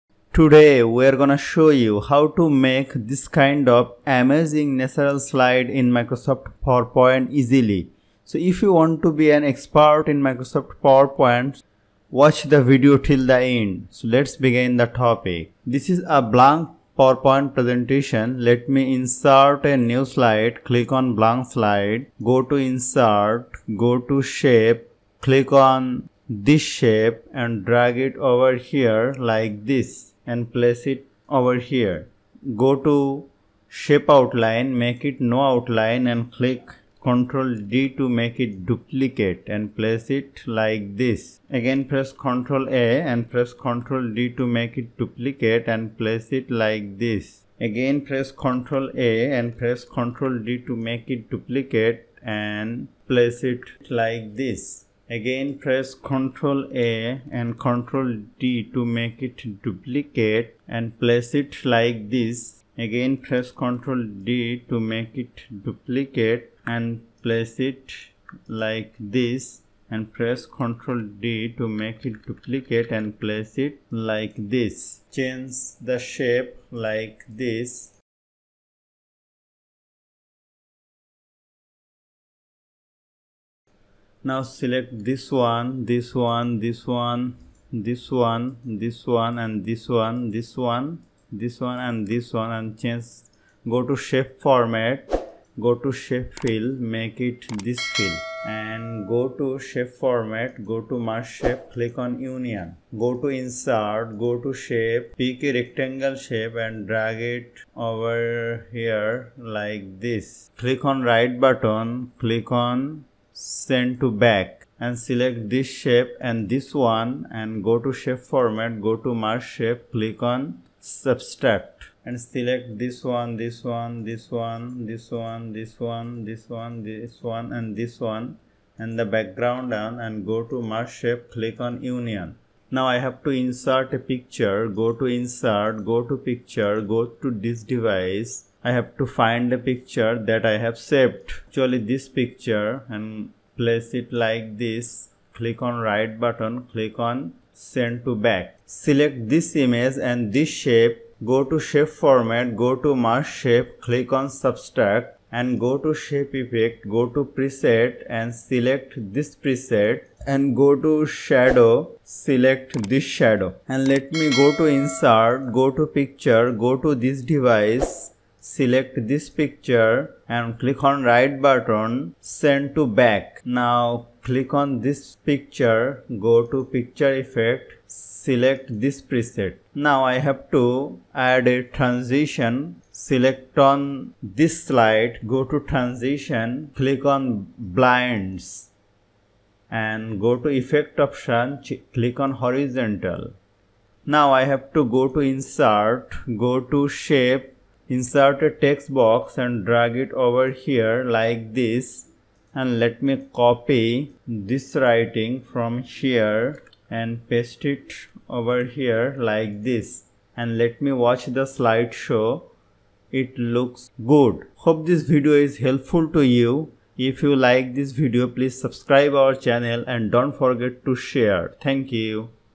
Easy way to design PowerPoint Nature Title slide
In This Tutorial, I Will discuss how to design a Stunning title slide in Microsoft PowerPoint. We will also discuss how to use transitions effect easily. After Watching This Video, you will Be an Expert in making amazing slides in PowerPoint with The Help of Online Education 360